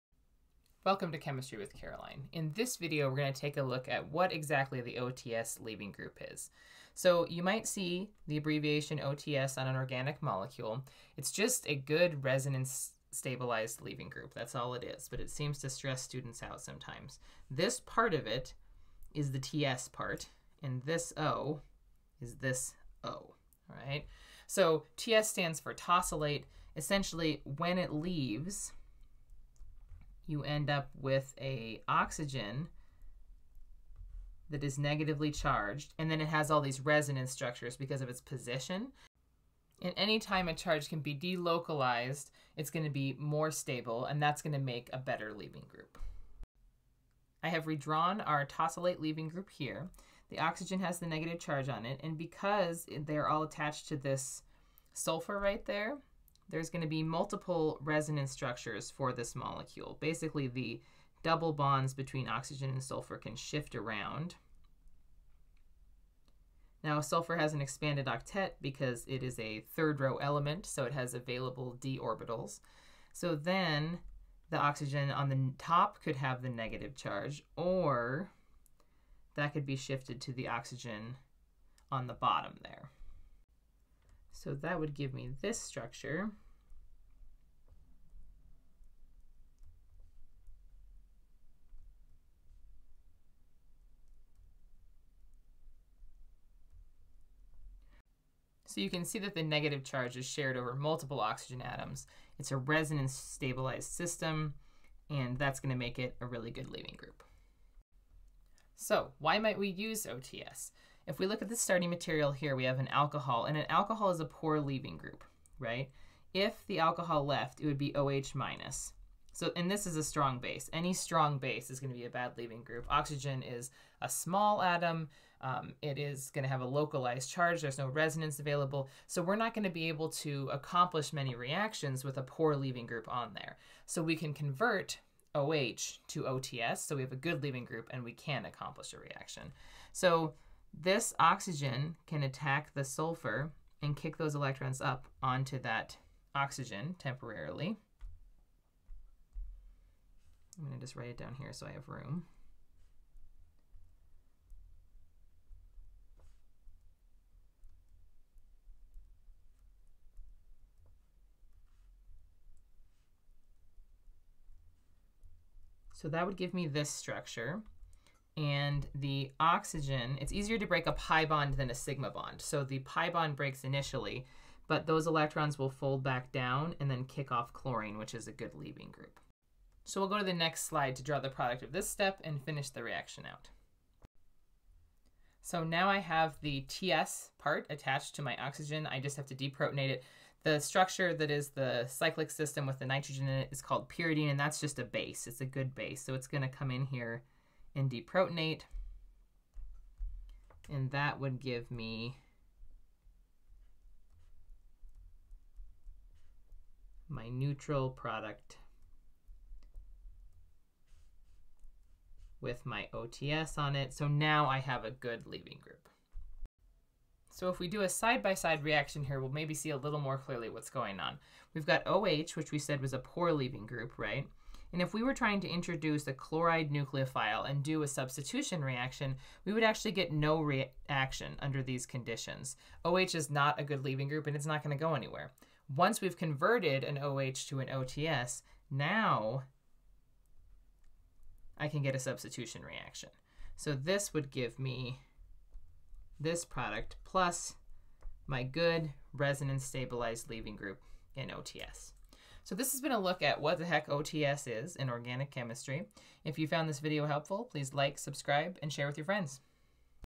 What is the OTs Leaving Group in Organic Chemistry?
In this video learn what what the heck the OTs leaving group is and why you should not panic when you see it.  Learn how to make an OTs leaving group to promote substitution reactions.